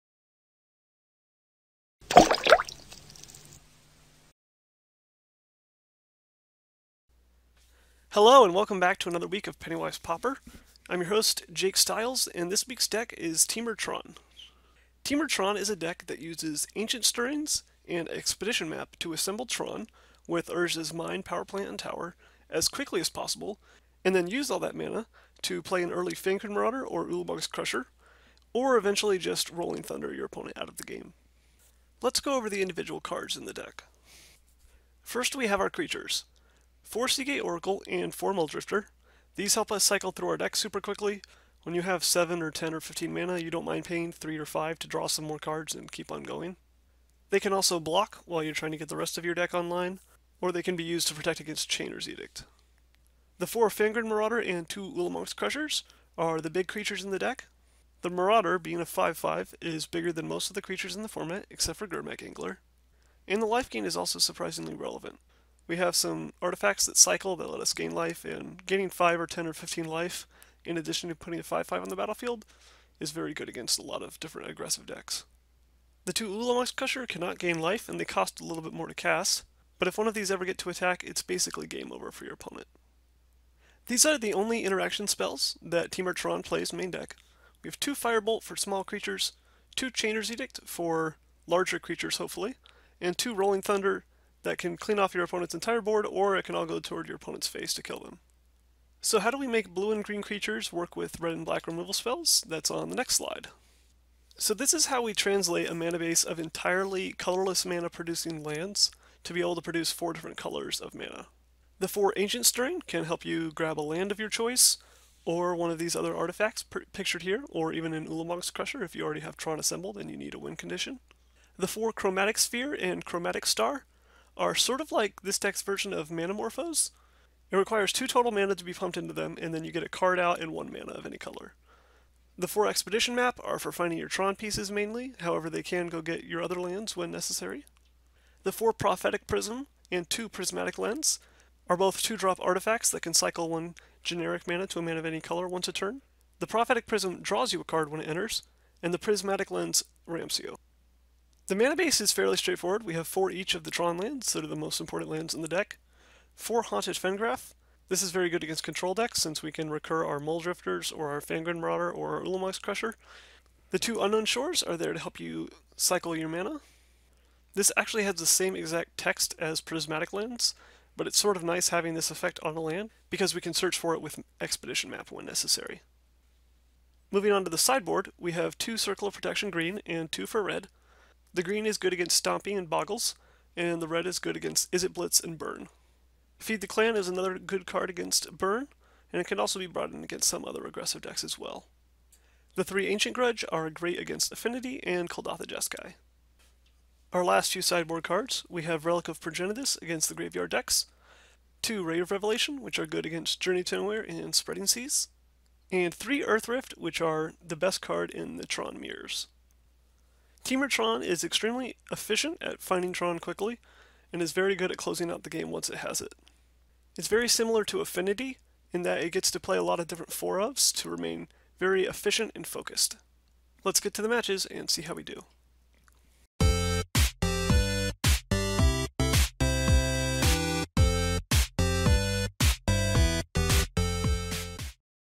Playing Pauper: Temur Tron Deck Tech
This Pauper deck boasts loads of mana and cantrips for days! Check out Temur Tron in action!